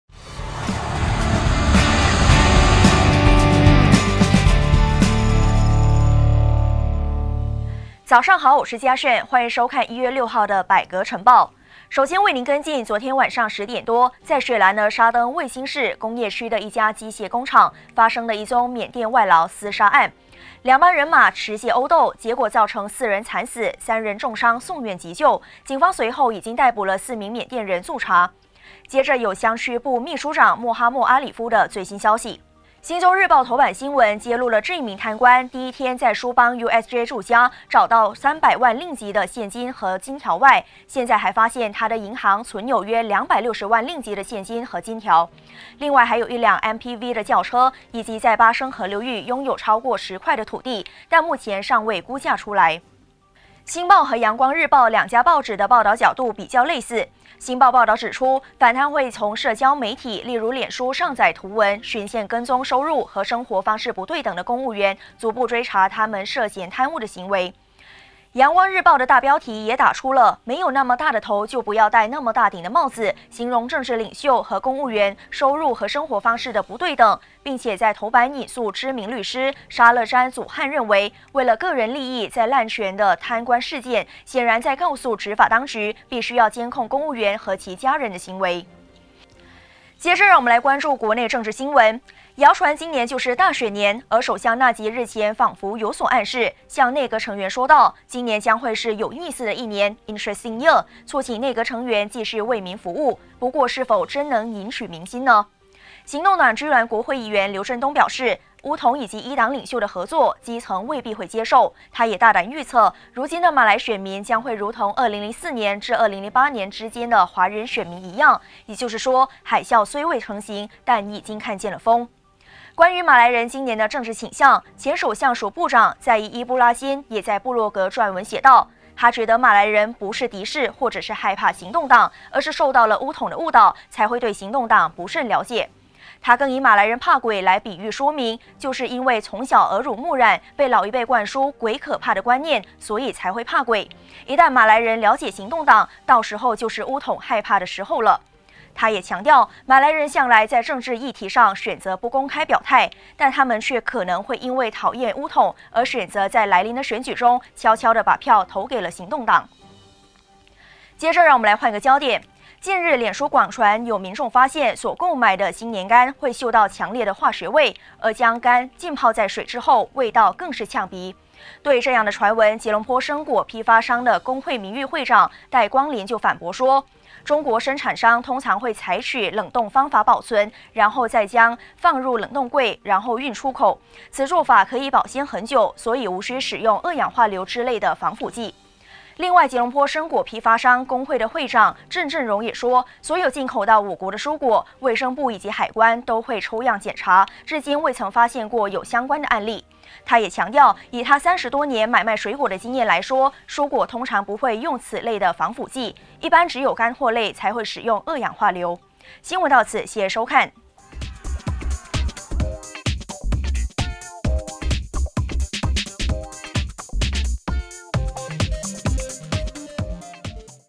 马来人怕行动党？再益以鬼喻巫统手法
大选或在今年上演，马来人政治倾向成为关注焦点。前首相署部长再益在部落格撰文写道，巫统如何影响马来人和行动党的关系。